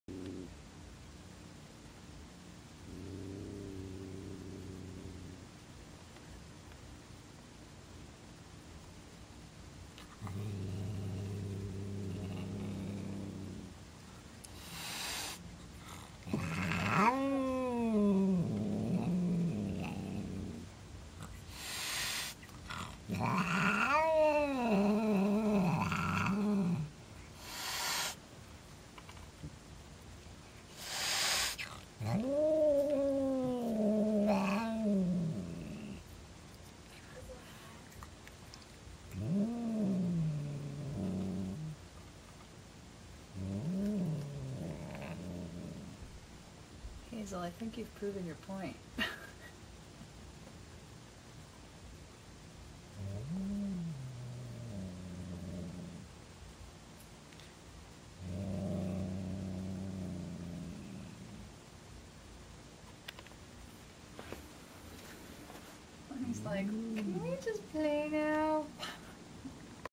Hazel Flips Out
My cat, Hazel, suddenly decided she hated her brother Lenny, after two years of relative peace.  She started hissing and growling at him yesterday.  He's a big dumb loving dope and just tries to get close to her, but she wants none of it.